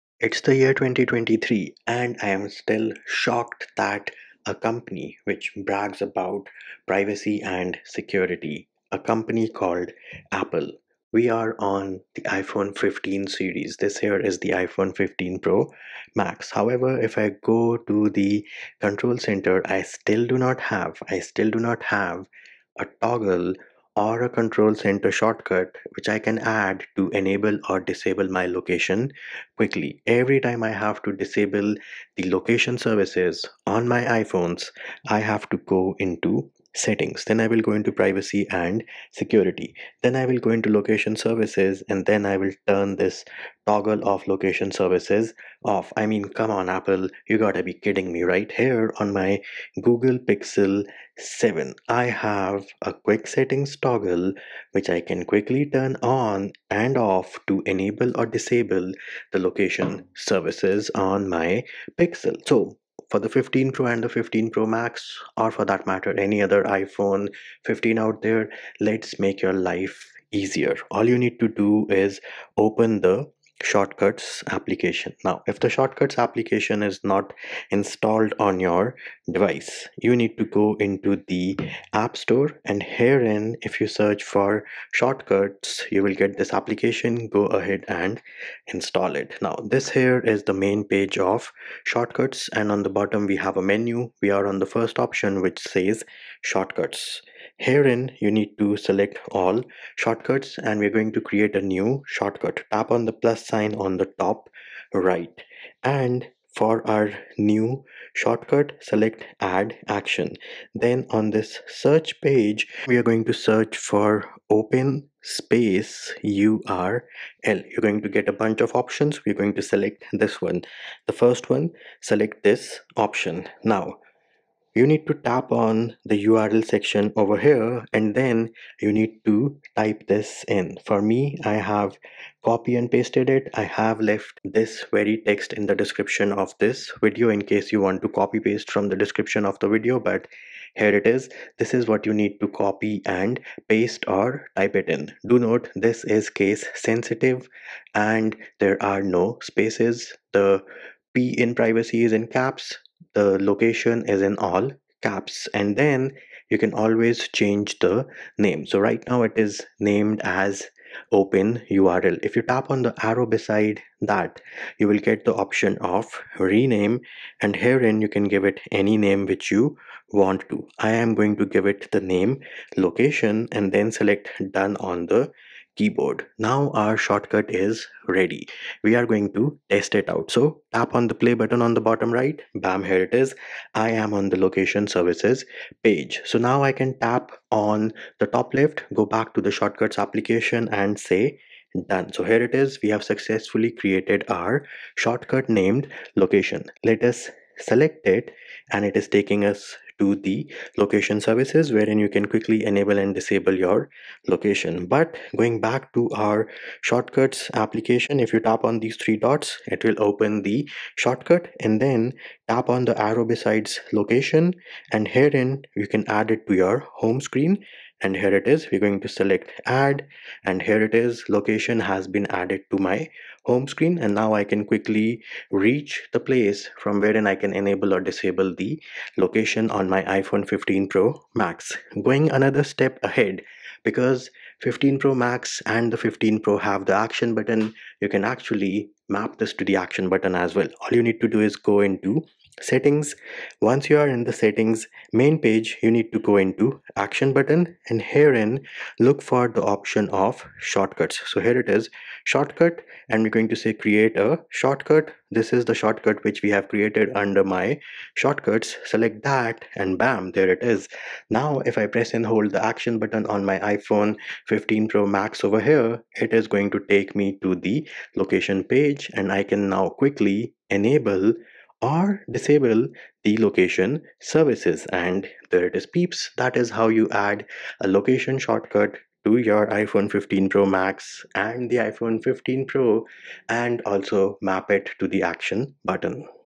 iPhone 15 Pro Max & iPhone 15 Pro Add Location Shortcut (On/Off) to Home Screen & Action Button!
Did you just get your hands on a iPhone 15 Pro Max or iPhone 15 Pro and want to learn how to add a location shortcut to the home screen and also map it to the Action button? In this detailed video tutorial we will show you how to add a location shortcut to the home screen on the iPhone 15 Pro Max and iPhone 15 Pro, and also map it to the Action Button.

*iPhone 15 Pro Max & iPhone 15 Pro How to add a location shortcut text*
Shortcut Text
prefs:root=Privacy&path=LOCATION